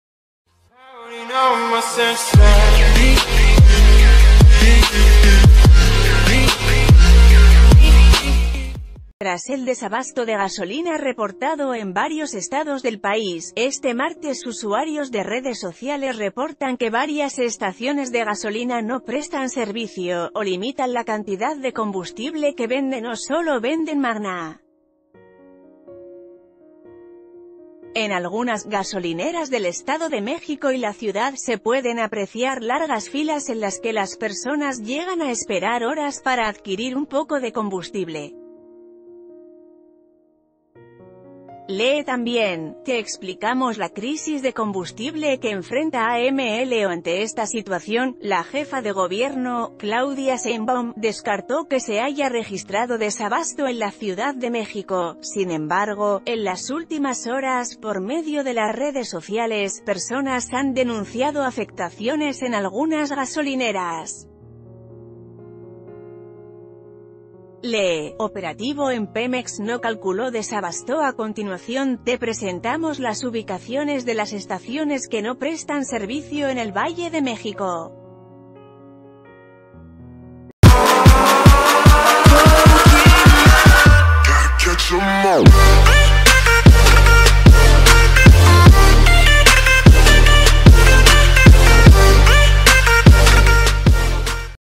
Estas son las estaciones de gasolina sin servicio en el Valle de México
Tras el desabasto de gasolina reportado en varios estados del país, este martes usuarios de redes sociales reportan que varias estaciones de gasolina no prestan servicio, o limitan la cantidad de combustible que venden o solo venden magna. En algunas gasolineras del Estado de México y la ciudad se p...